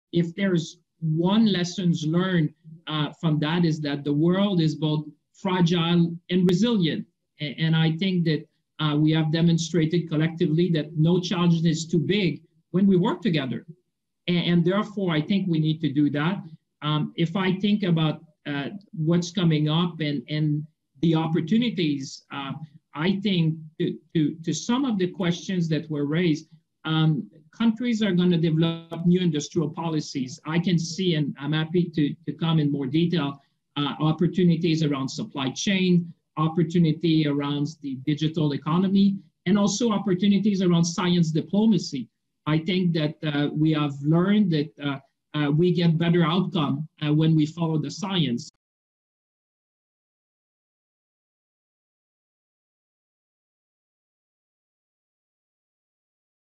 François-Philippe Champagne | The World is Fragile and Resilient
Speaking at Davos Agenda Week 2021 during Resetting Geopolitics (Option 2).